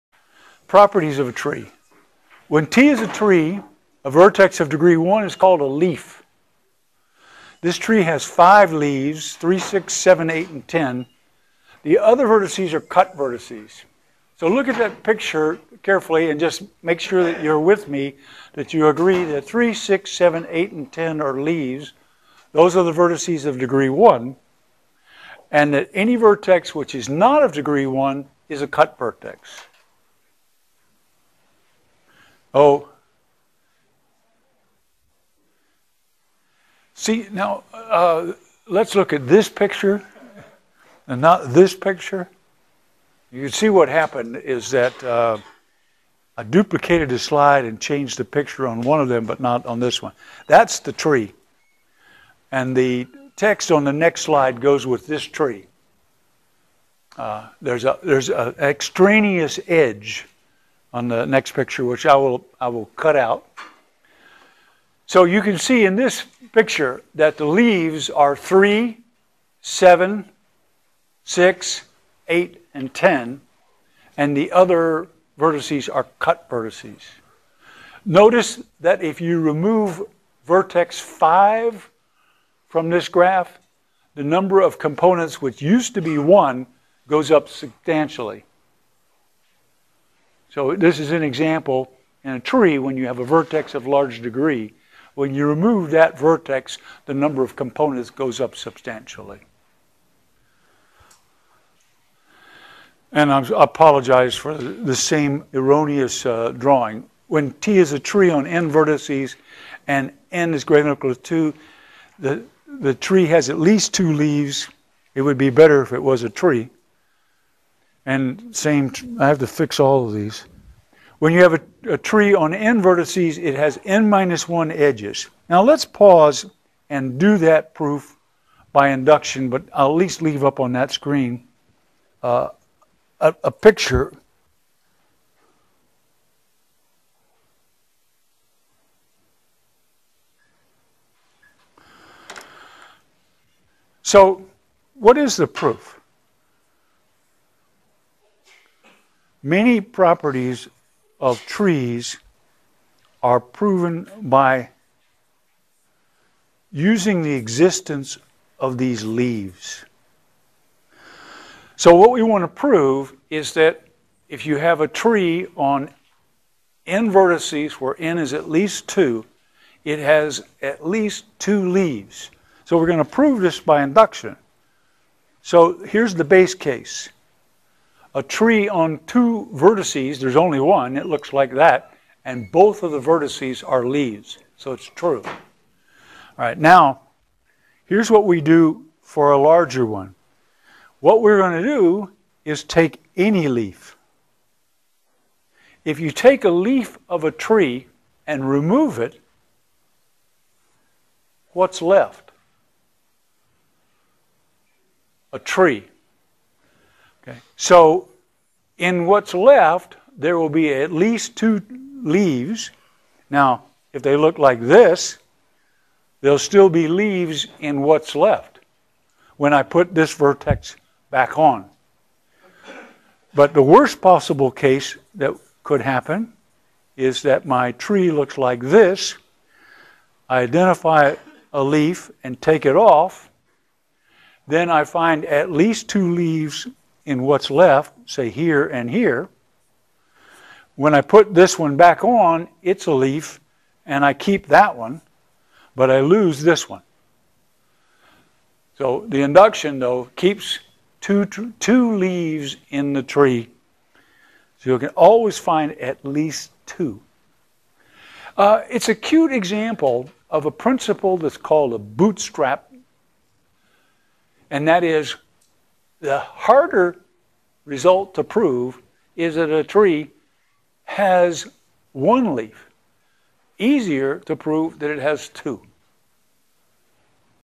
L07V03 Special Classes of Graphs
This video defines and provides a few examples of special classes of graphs (cycles, complete graphs, cliques, trees).